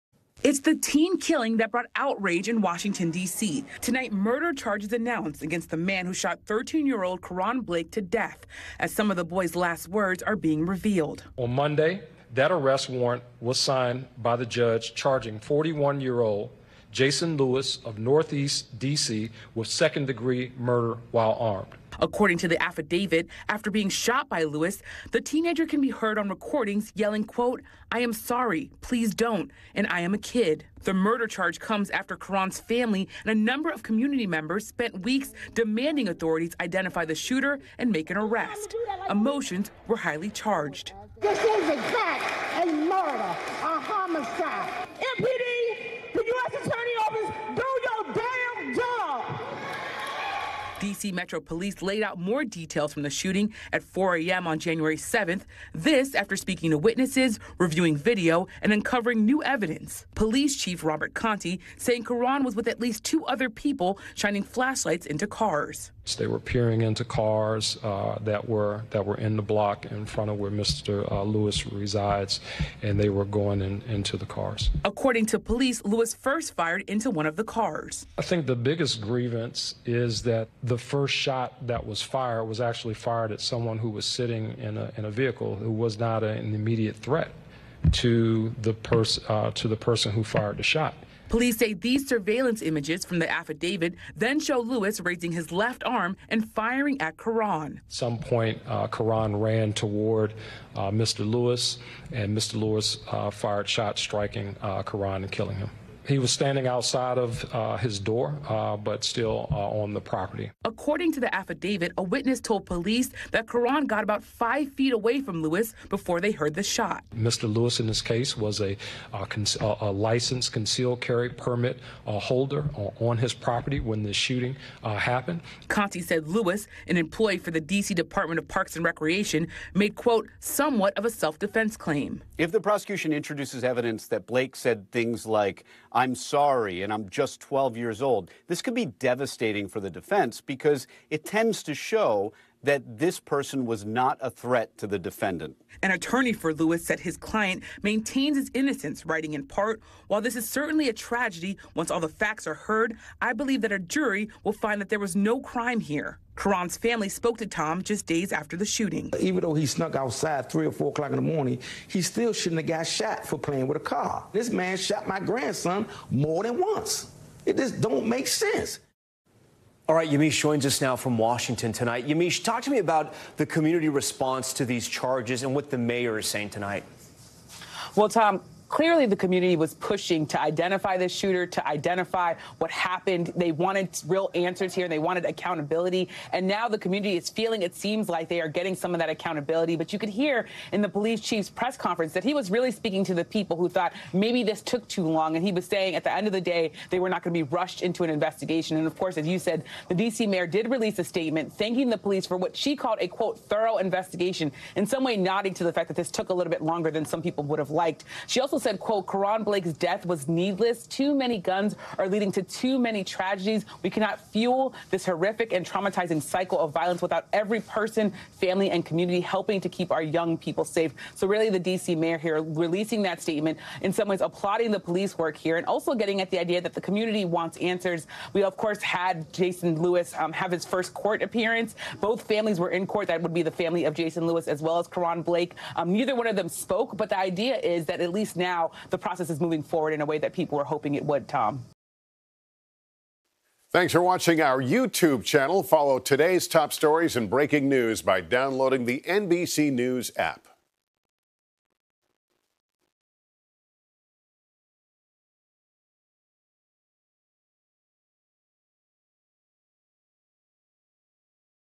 Washington, D.C., city employee charged in fatal shooting of 13-year-old
Washington, D.C., prosecutors charged a city official with second-degree murder in the fatal shooting of 13-year-old Karon Blake earlier this month. NBC’s Yamiche Alcindor has the latest on the investigation as authorities reveal the teen shouted “I am a kid” to his alleged killer.